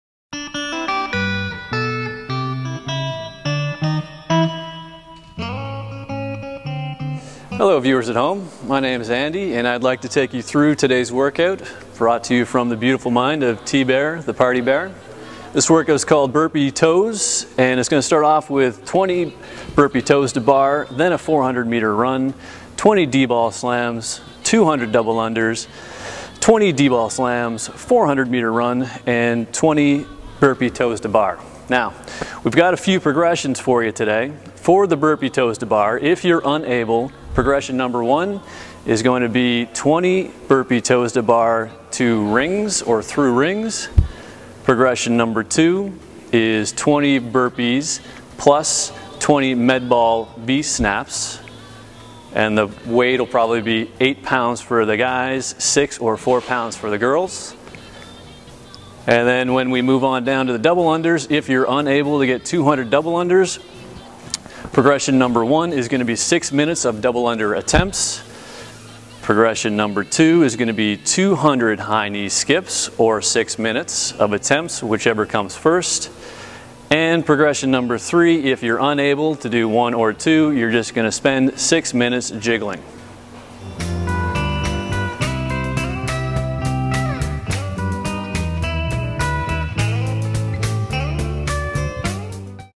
'Burpee Toes' Progressions
Andy demos some interesting progressions for TBear's 'Burpee Toes' Workout.